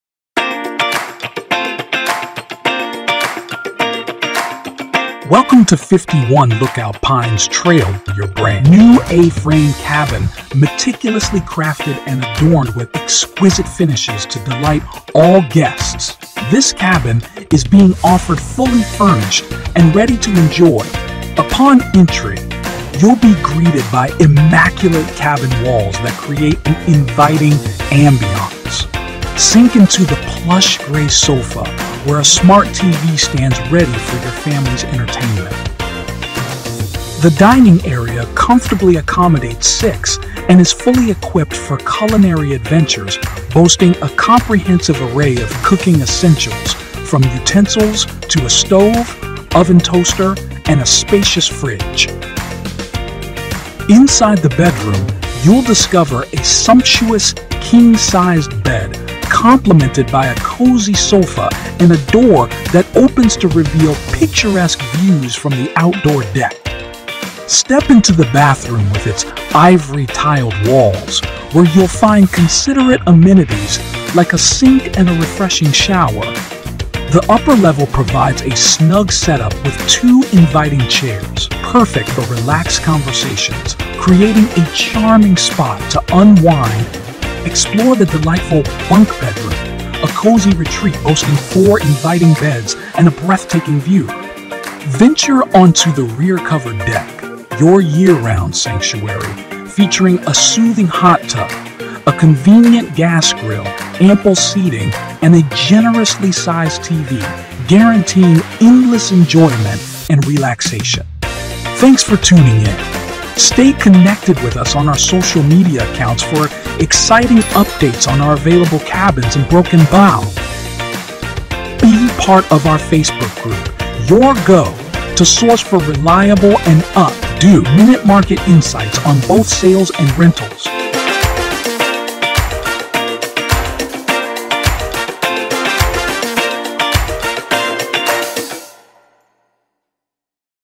Just Listed! 53 Lookout Pines | Broken Bow Luxury Cabin
Welcome to Lookout Pines, a beautifully decorated cabin designed for your comfort and enjoyment. This spacious retreat comfortably accommodates up to 6 guests, with a master bedroom downstairs and two sets of cozy bunks upstairs.

One of the highlights of Lookout Pines is the covered deck in the back, where you can relax in the hot tub, fire up the gas grill, and unwind in ample seating while watching your favorite shows on the large TV, no matter the season!

This cabin is a turn-key opportunity, fully furnished and ready for you to make it your own. It's conveniently located in the heart of Hochatown, ensuring you're close to all the attractions and activities this area has to offer. Secure your stay at Lookout Pines today and experience the perfect cabin getaway!